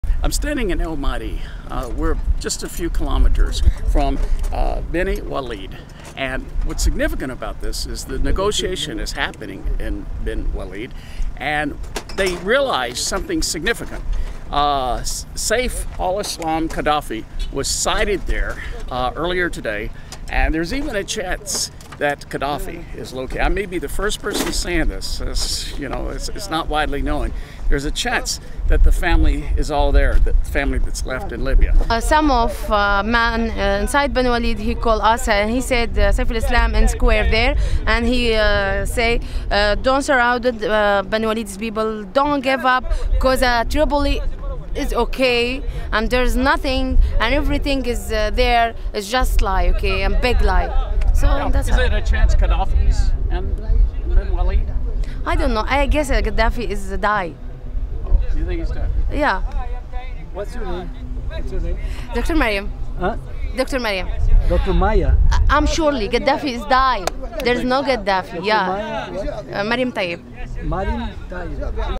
I'm Chasing Saif al-Islam Gaddafi Down in Bani Walid!
Bani Walid was one of the last strongholds of Gaddafi loyalist. In Sept. 2011 I visited just outside of that town as we had heard Saif, Gaddafi's son was there. This town also has a small airport so that made sense. I also got to interview a female dentist/soldier who had just left that town and had seen and heard Saif say that everything is ok Libya, Tripoli is just like here, we (Gaddafi family) are still in control! A lady at our event Sunday with the Libyans in Dallas asked me if I had any of the female soldiers of Libya who played such a great role in the revolution.